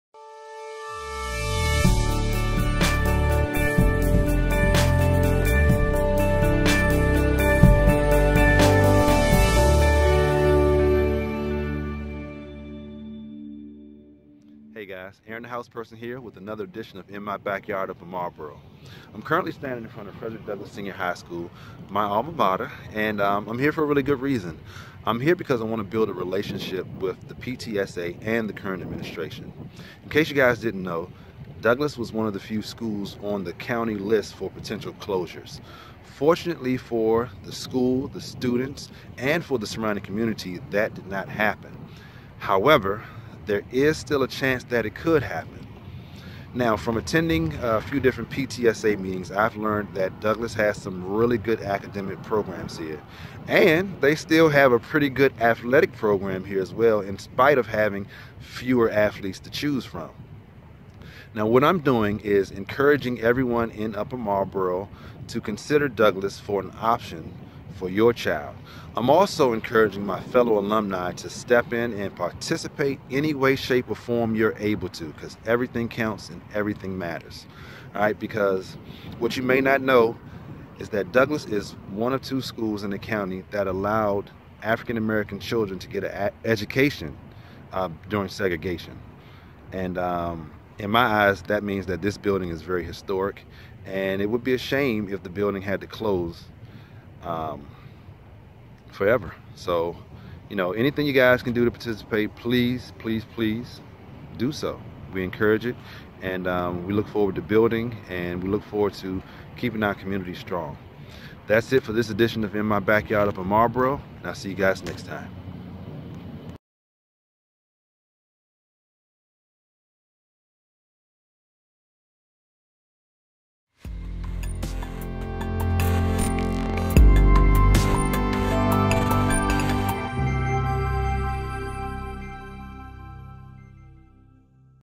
In My Backyard Upper Marlboro | Frederick Douglass | 20772
In this episode of In My Backyard Upper Marlboro, MD - I'm visiting my Alma Mater in order to build relationships and spread awareness about the schools potential closing & it's historic relevance.

Realtor associated with

EXIT Bennett Realty
7701 Greenbelt Road  #100
Greenbelt, MD 20706